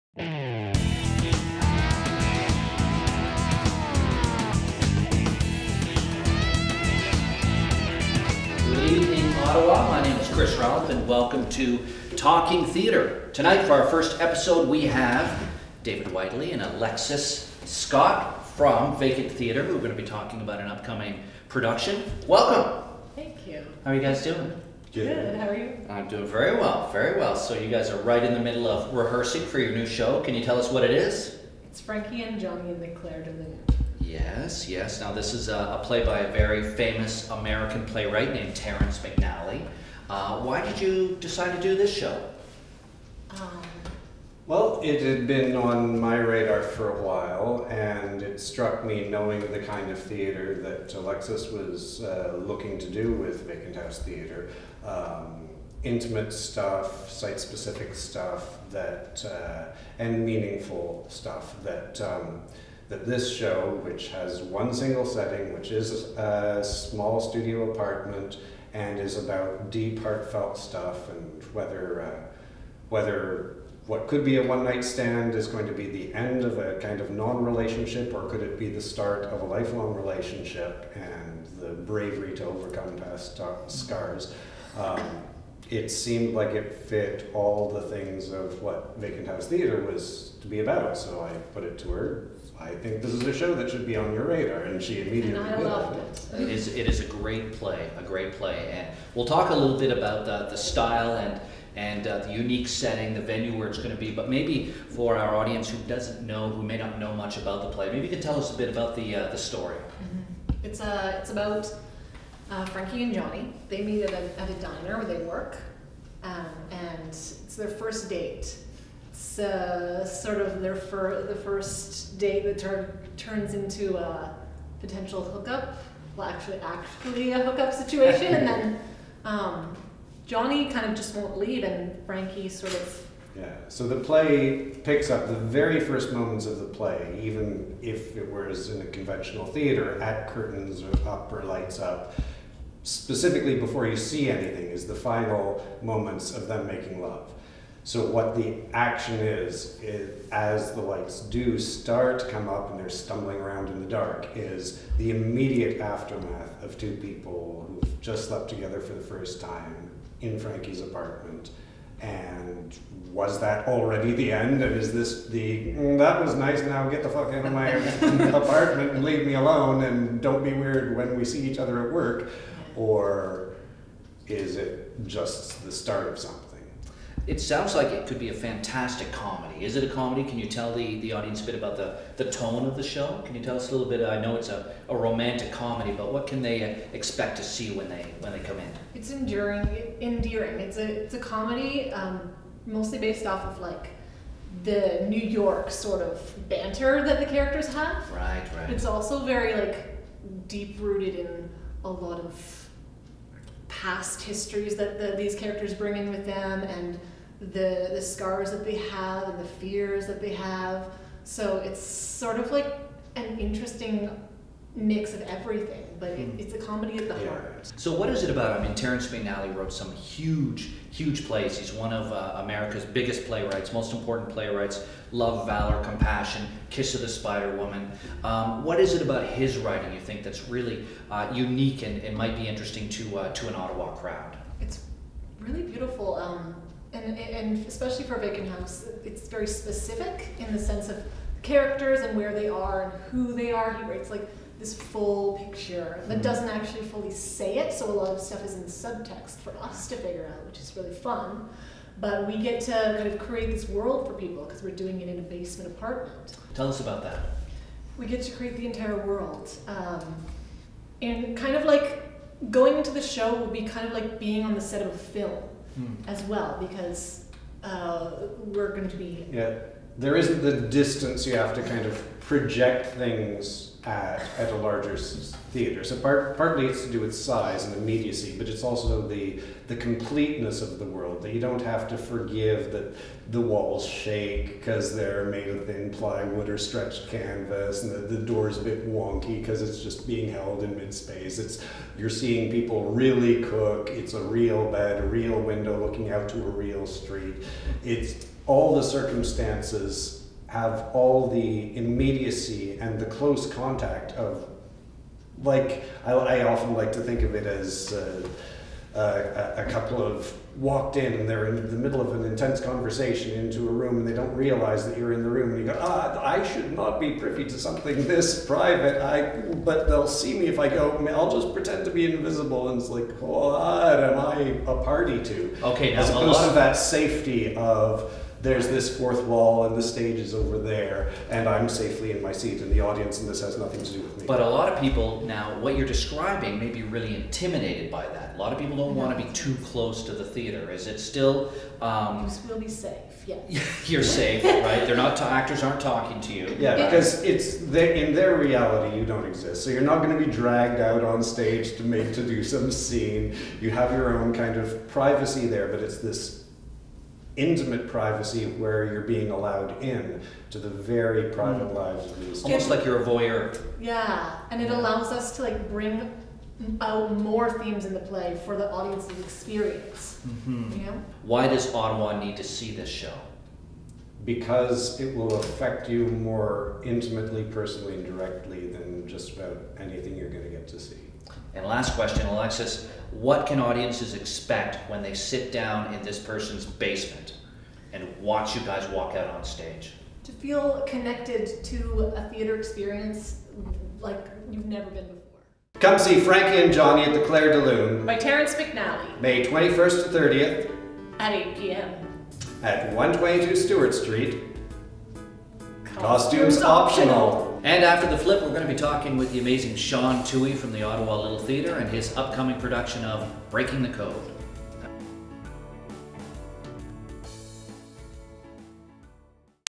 Theatre Talk: Frankie and Johnny in the Claire de Lune
All your local theatre coverage @ onstageottawa.com

ottawalittletheatre.com (Breaking the Code, opening May 5th)
vacanthousetheatre.com (Frankie and Johnny in the Claire de Lune, opening May 21st)
thegladstone.ca (The End of Civilization, opening May 15th)
kanatatheatre.com (The Black Bonspiel of Wullie MacCrimmon, opening May 12th)
artscourt.ca (Up to Low, opening May 21st)

Each segment link.

-- David Whitely and Alexis Scott tell us about their upcoming production of Frankie and Johnny in the Claire de Lune

-- Shaun Toohey tells us about Ottawa Little Theatre's upcoming production of Breaking the Code

-- Shaun Toohey, David Frisch, and Mary Ellis sit around and talk about beginnings and pick up lines.

-- David Frisch and Mary Ellis tell us about the upcoming production of The End of Civilization

-- Janet Irwin tells us about her upcoming production of Up to Low

-- Paul Benhcke and Rom Frigon tell us about Kanata Theare's production of The Black Bonspiel of Wullie MacCrimmon

-- Janet Irwin, Paul Benhcke, and Rom Grifon sit down and talk about superpowers and annoying habits.

SHOW CREDITS:

On Stage Ottawa's Talking Theatre is produced by Allan Mackey, Valley Wind Productions (valleywind.com).
The show is hosted by Chris Ralph and shot on location in the Avalon Studio thanks to the support of the Acting Company (actingcompany.ca).

Floor crew, lighting & camera:
Sabrina Betts, Jessica Mardyla